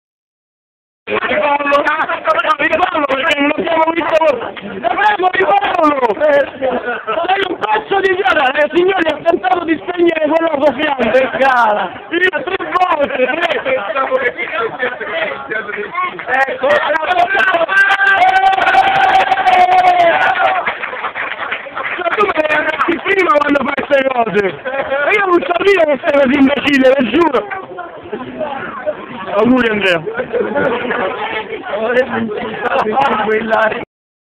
andrea soffia la candela, ma...
senza parole, non ho mai visto nessuno tentare di spegnere un fuoco d'artificio col fiato!